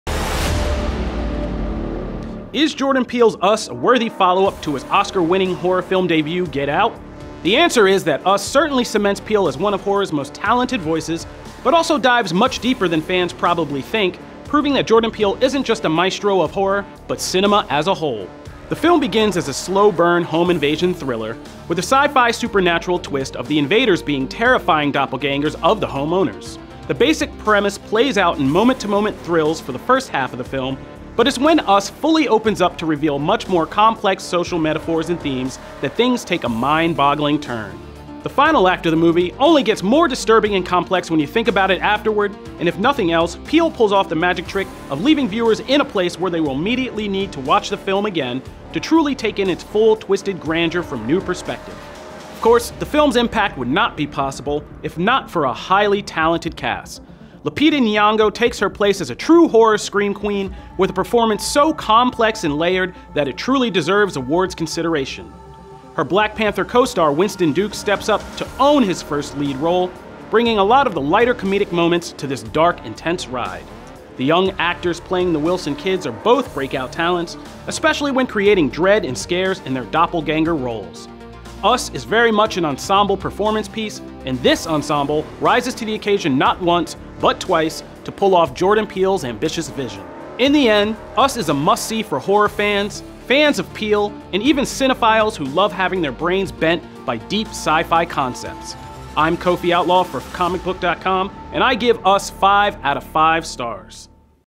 Us - ComicBook Movie Review
🌟🌟🌟🌟🌟 Jordan Peele’s 'Us' is the PERFECT follow-up to his Oscar-winning ‘Get Out’! FULL REVIEW